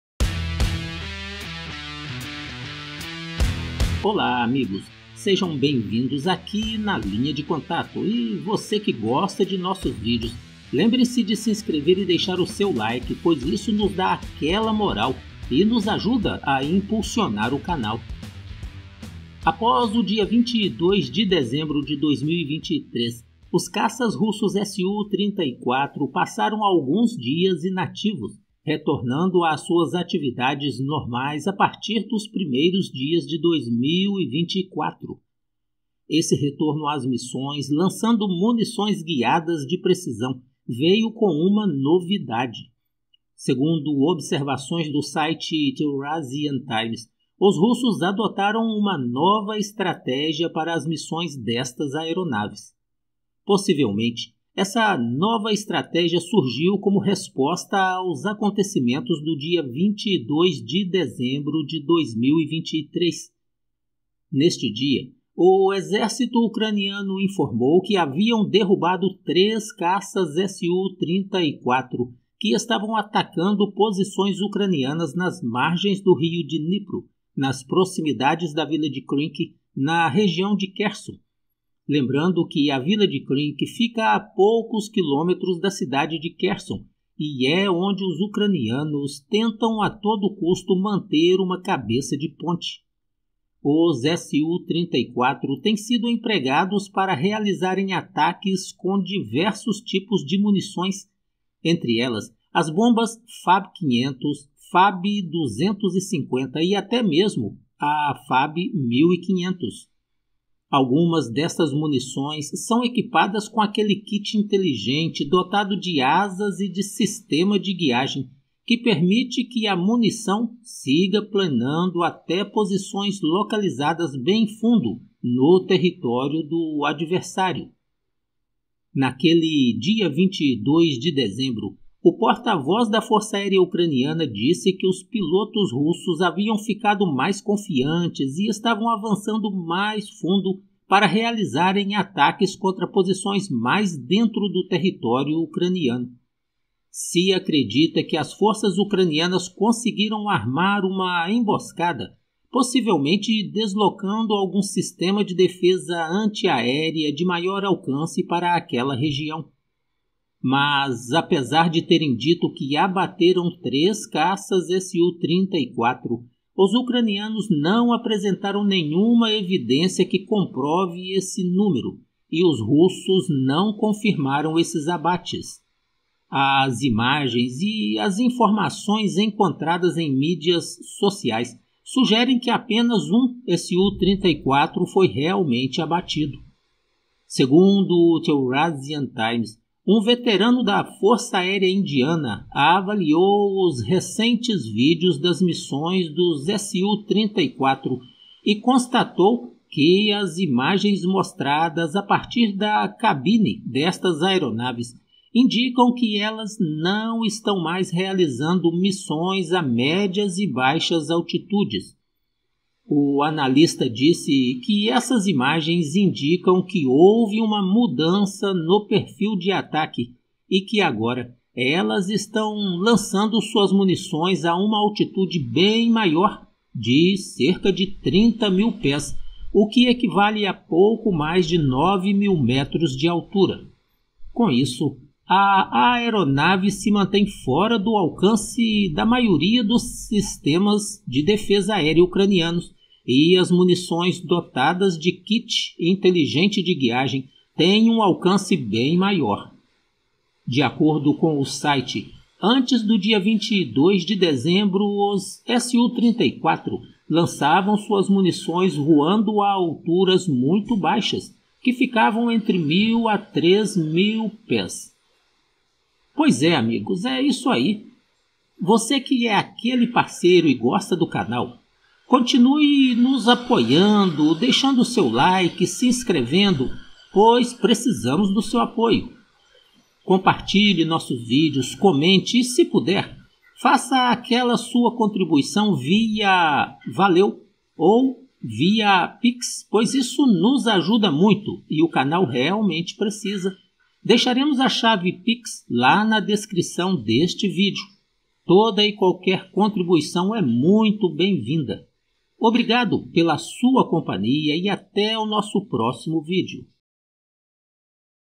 Rússia mudou a estratégia de emprego dos caças Su-34 Para atingir alvos mais longe
Chave pix:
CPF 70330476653

Assistan curta, comente e compartilhe:
Acredito que você irá gostar.